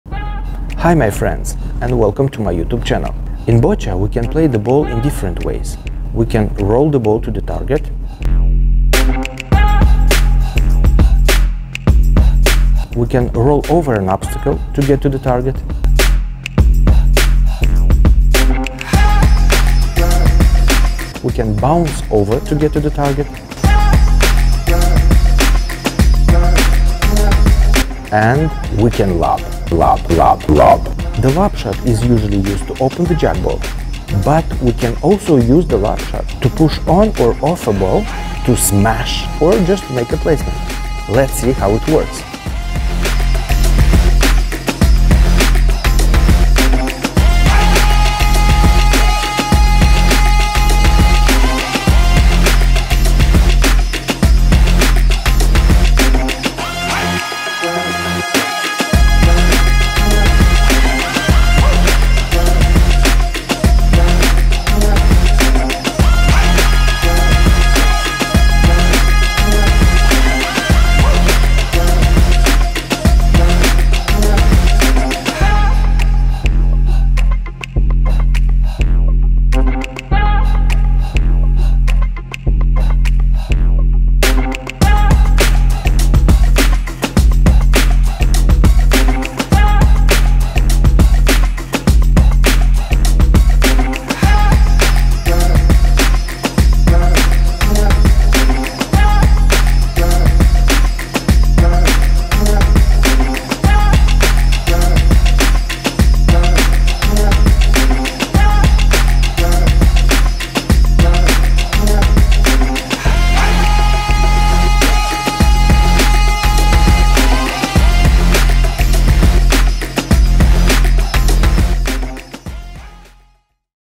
Lob shot (Boccia)
Lob shot - An aerial shot played to move a target ball by attacking over a ball (BISFed OFFICIAL TERMINOLOGY).